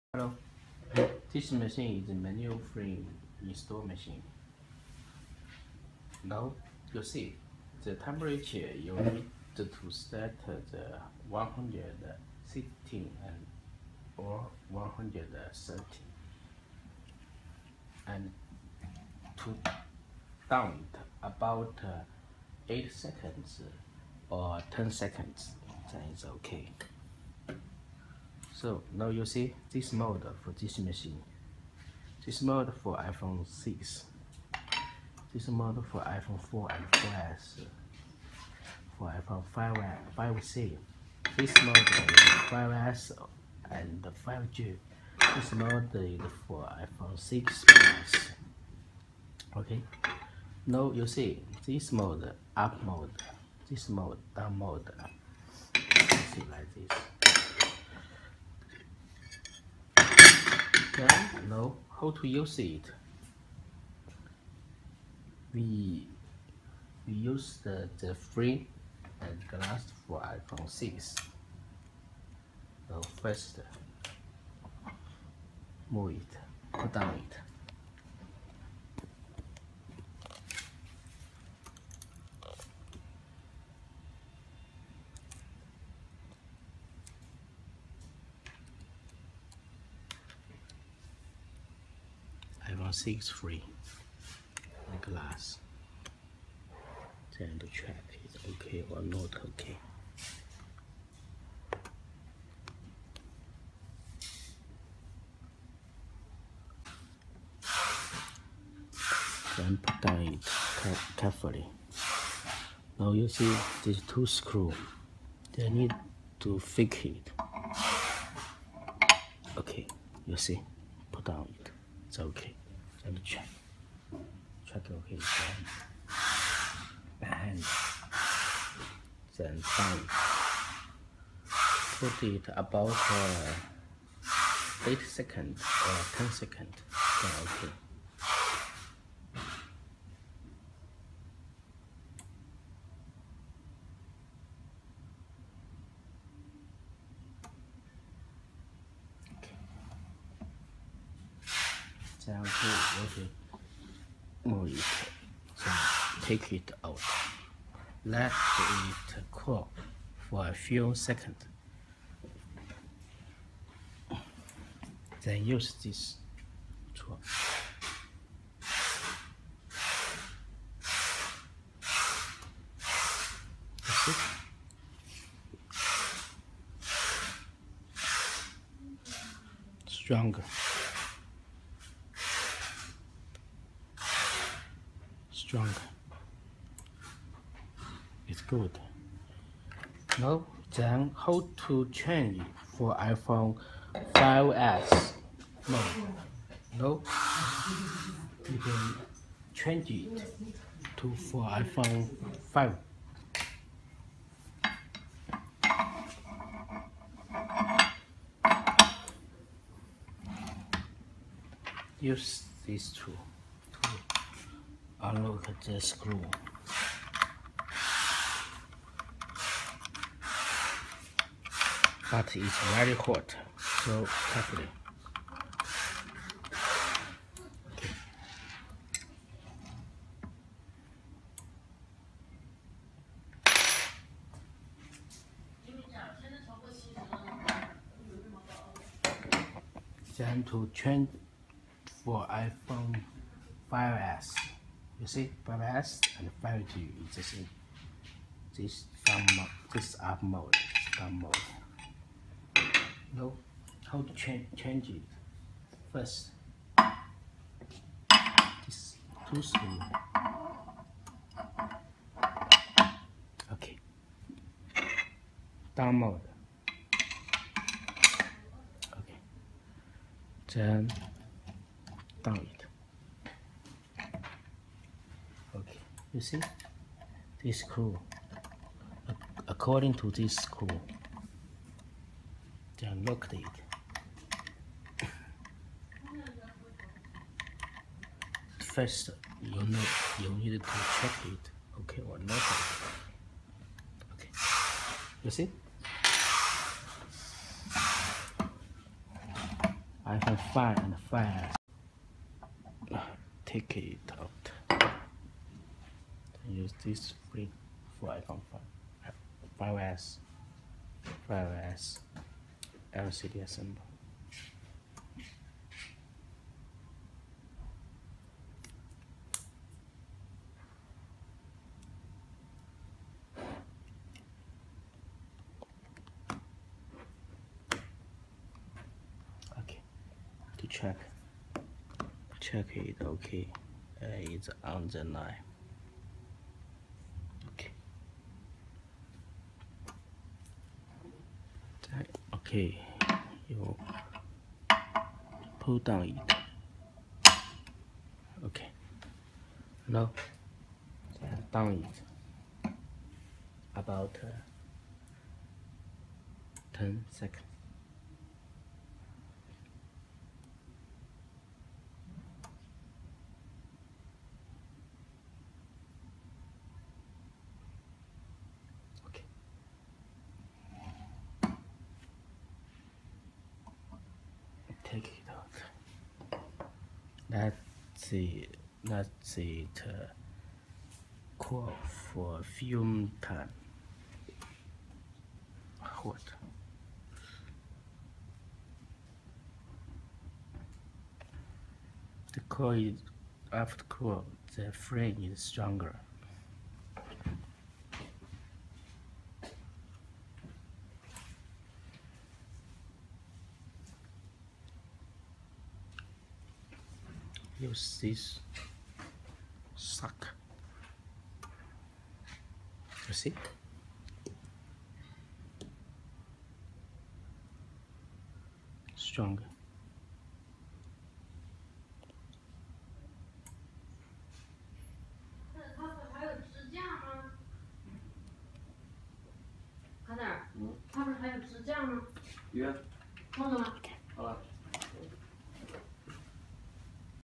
How to use & chang mold for Manual frame install machine frame laminating machine
1.operate easily
2.Good price
3.Mold: for iphone 4;for iphone 5&5S ;For iphone 5c;for iphone 6 ;for iphone 6 plus